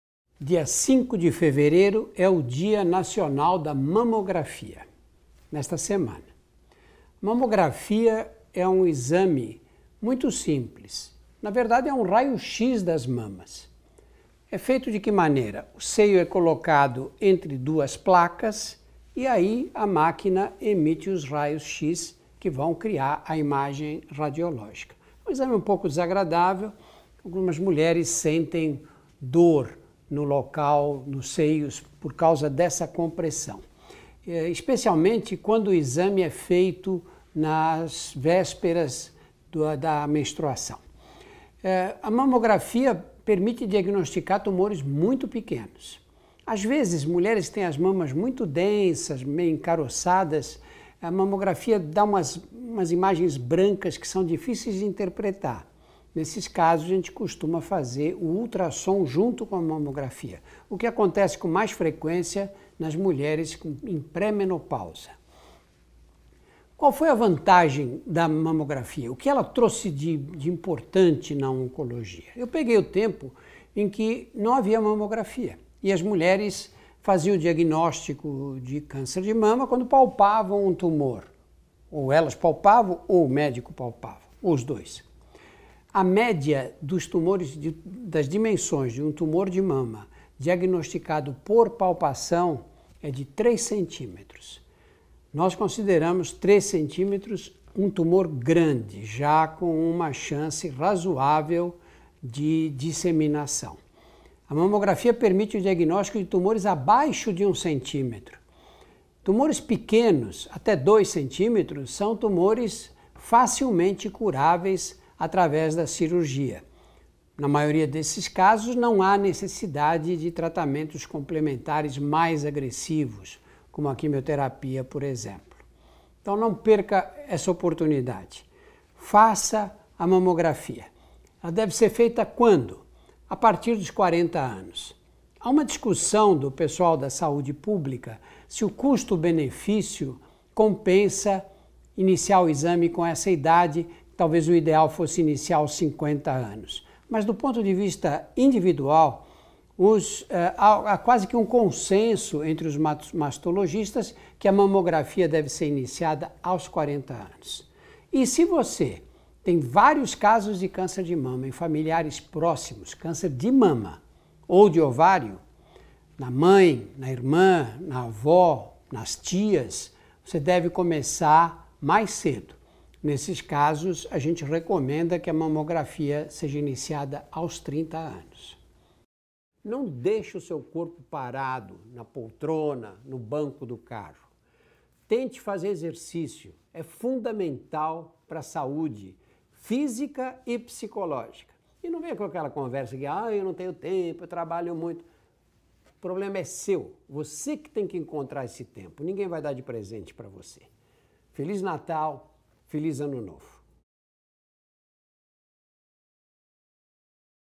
Mamografia | Coluna #18
A mamografia é o único exame diagnóstico capaz de detectar o câncer de mama. Assista ao recado do dr. Drauzio.